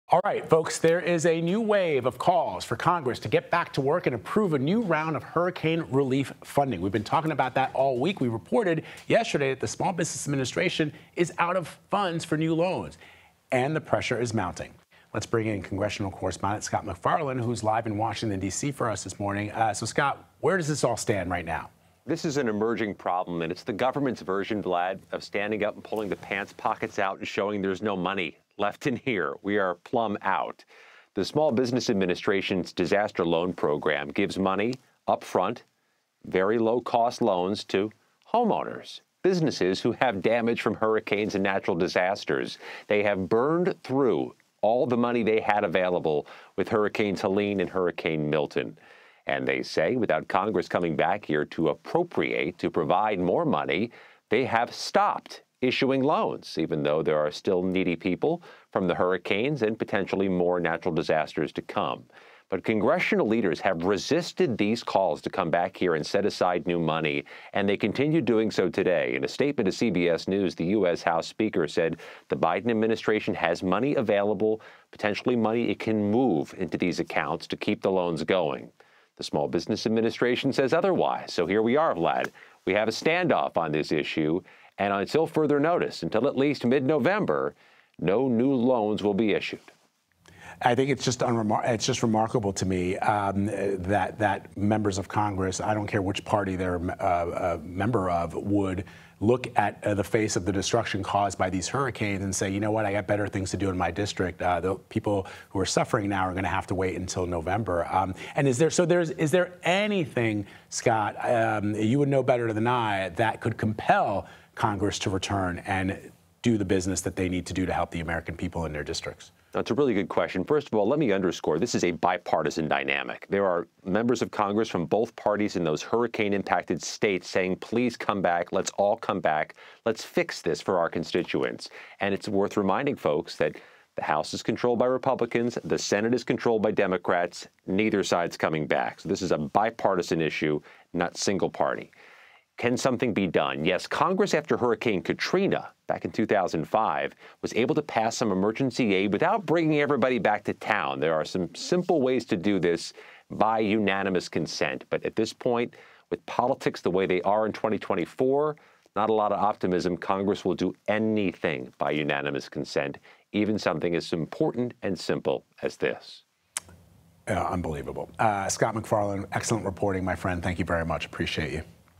New calls for Congress to return from recess for hurricane relief funding
There is a new wave of calls for Congress to come back from recess and approve a new round of hurricane relief funding with the Small Business Administration out of money for new disaster relief loans. CBS News congressional correspondent Scott MacFarlane has more.